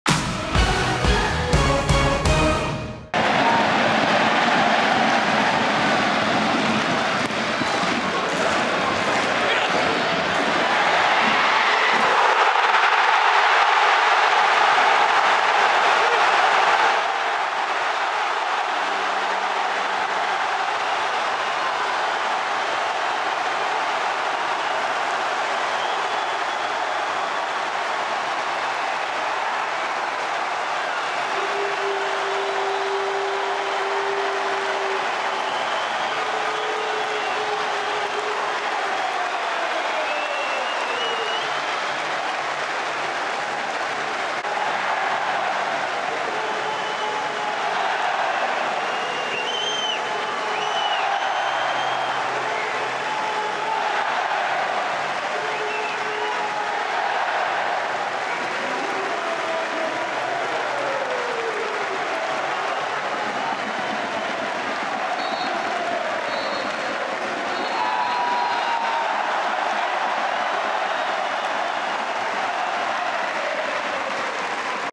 Goal Romain FAIVRE (90' +8 - FCL) FC LORIENT - AS MONACO (2-2) 23/24
"FC LORIENT - AS MONACO (2-2) Romain FAIVRE (90' +8)  goal. FC LORIENT vs AS MONACO Goals in video

Ligue 1 Uber Eats - Season 2023/2024 - Week 5 STADE YVES-ALLAINMAT - Sunday 17 September 2023